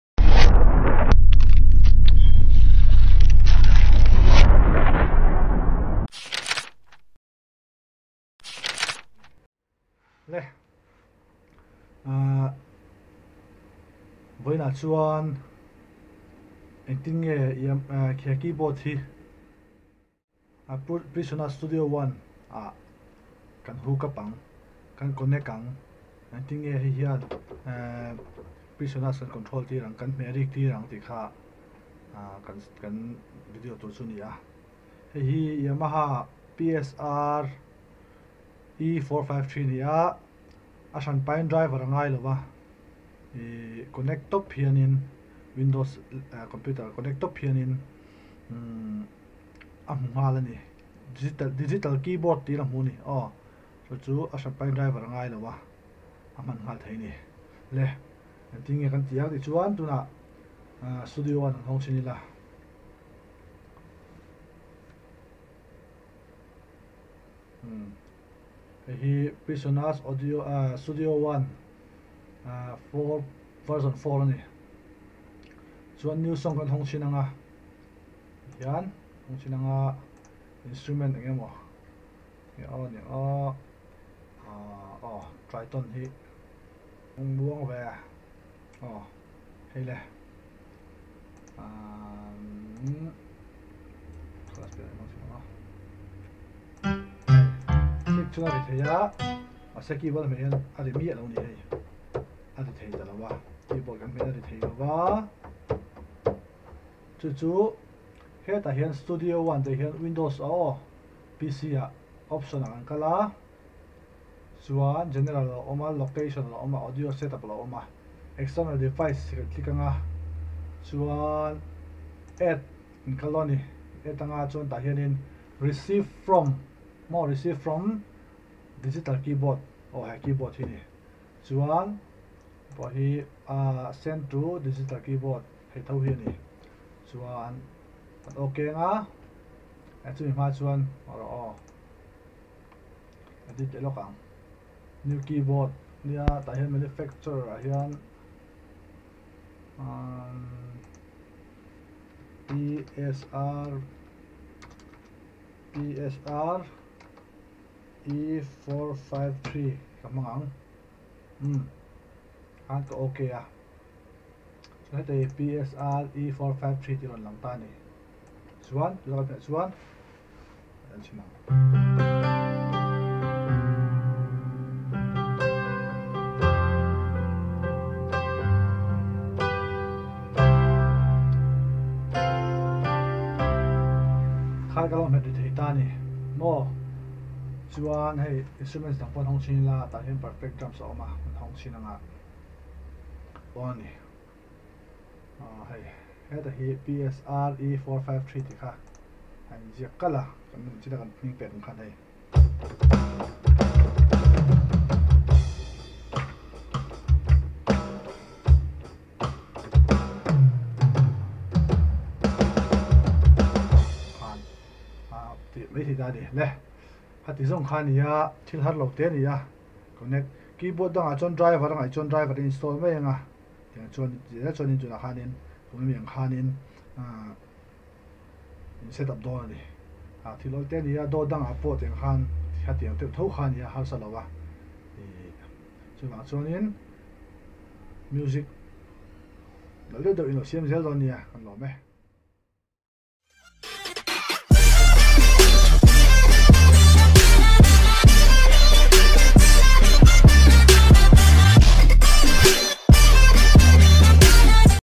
PSR E 453 | Studio One 4.6 tutorial | How to use Yamaha Keyboard as Midi Keyboard
Studio one a Keyboard setup dan